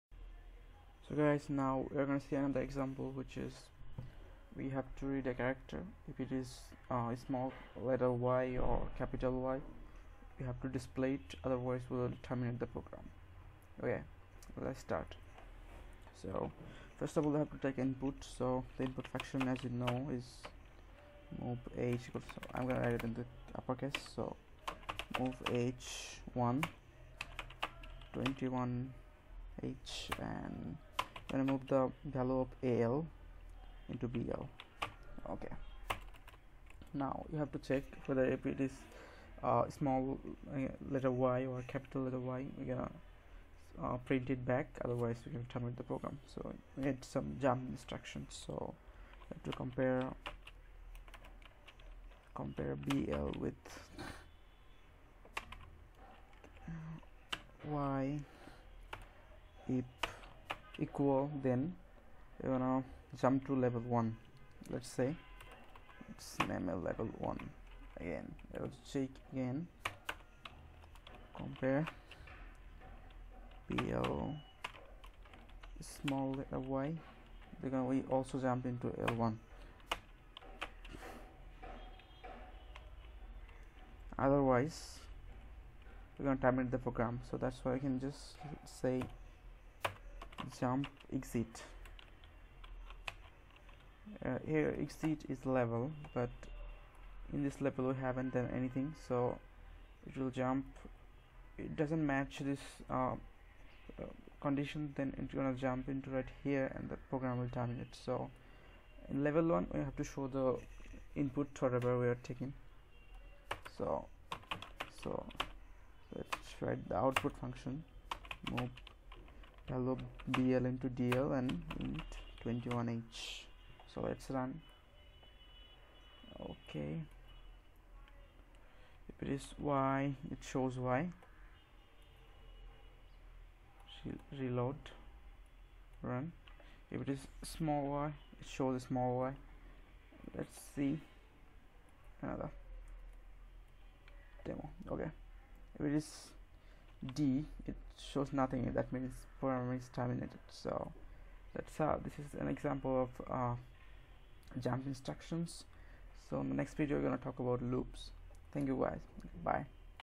13. COMPARING A SPECIFIC CHARACTER WITH INPUT|| Assembly Language Programming || English || emu8086
;Read a character. If it's "y" or "Y",
;display it; otherwise, •. terminate the program.

.MODEL SMALL
.STACK 100H
.CODE

MAIN PROC
    MOV AH,1
    INT 21H
    MOV BL,AL

    CMP BL,'Y'
    JE L1

    CMP BL,'y'
    JE L1

    JMP EXIT

    L1:

    MOV AH,2
    MOV DL,BL
    INT 21H

    EXIT:
    MOV AH,4CH
    INT 21H
    MAIN ENDP
END MAIN